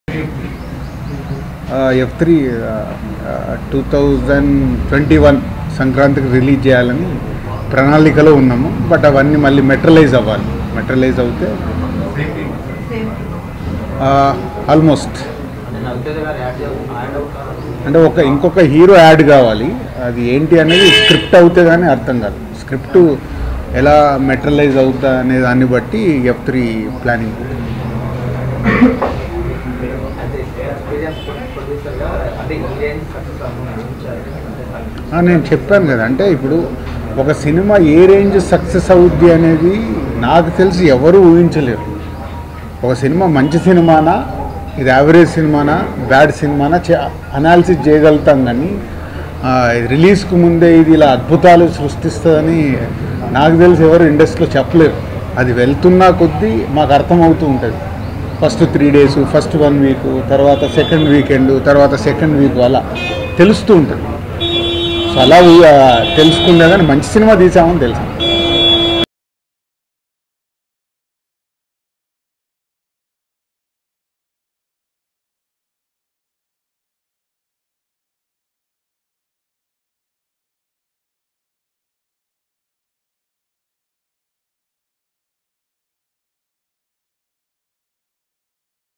Dil Raju Clarifies on Ravi Teja in F3 Movie with Venkatesh and Varun Tej
Watch Dil Raju Clarifies on Ravi Teja in F3 .